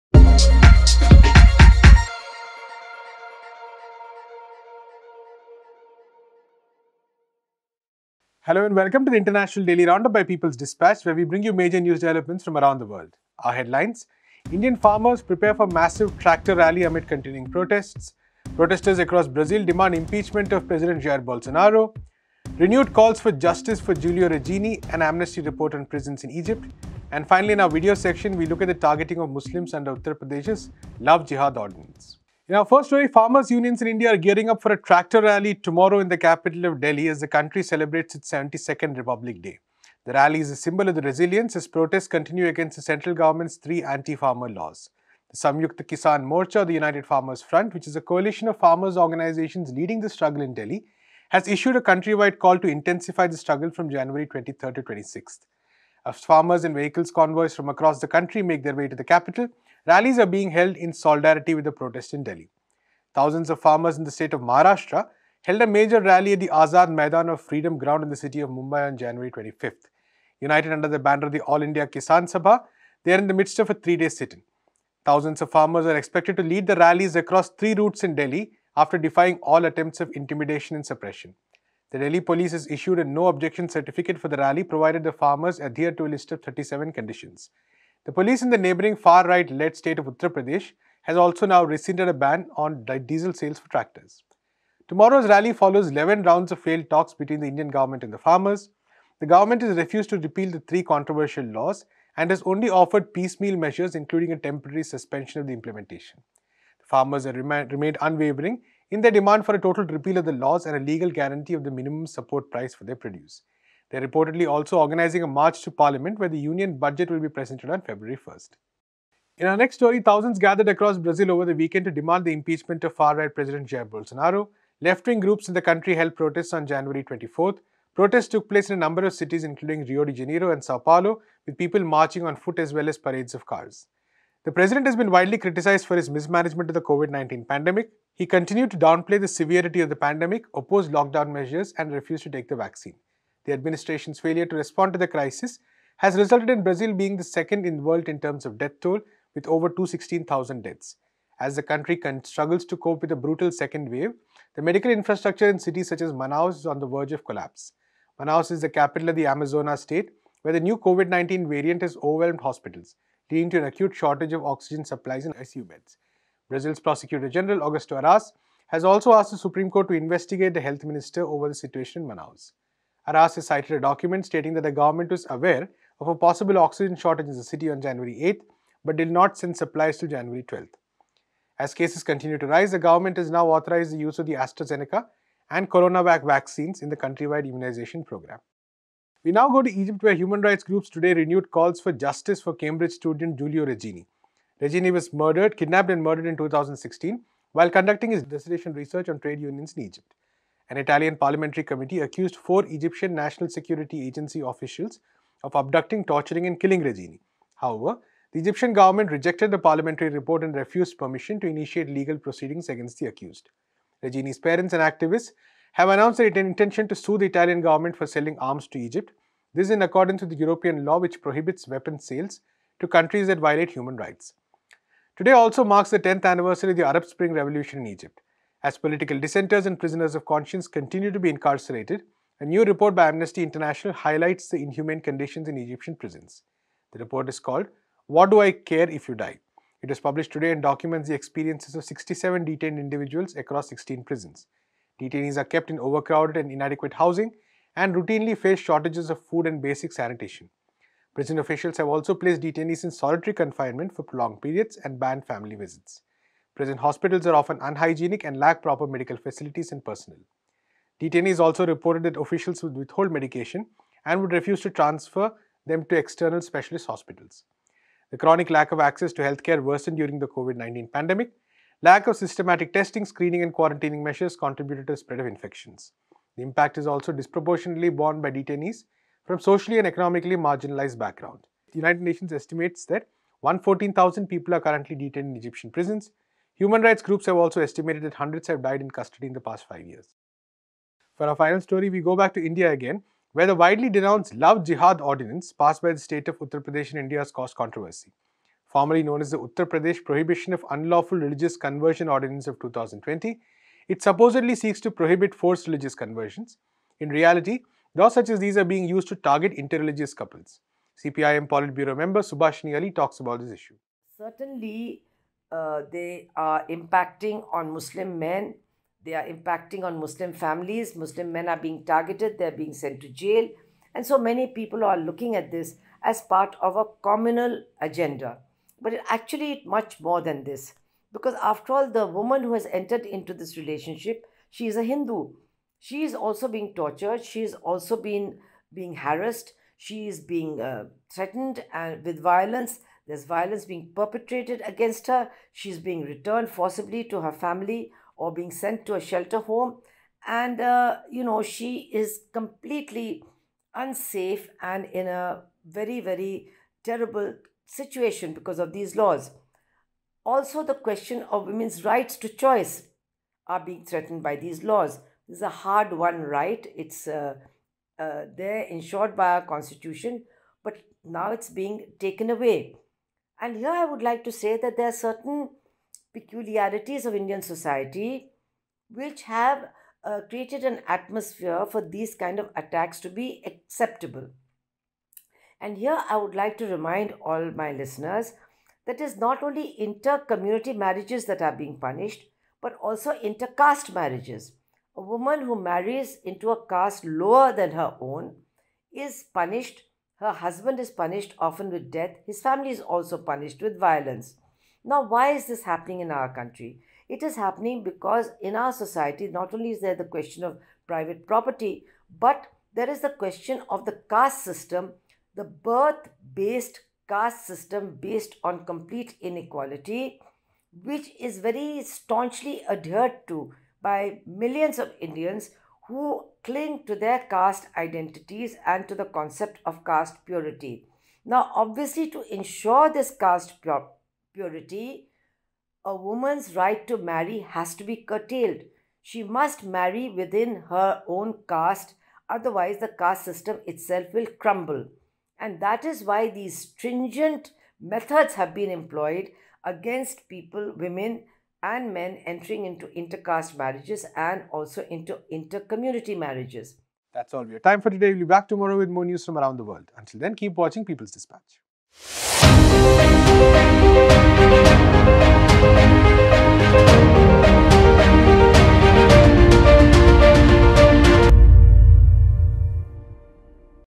Daily Round-up| Indian farmers gear up for tractor rally on Republic Day and other stories
In today’s episode of the Daily Round-up we look at the preparations for the Tractor Parade organized by the farmers protesting at the borders of Delhi, the protests in Brazil calling for the impeachment of President Jair Bolsonaro, the renewed calls for justice for Guilio Regini  and an Amnesty report on the  conditions of Egyptian prisons, and the ‘Love Jihad’ ordinance introduced in the Indian state of Uttar Pradesh.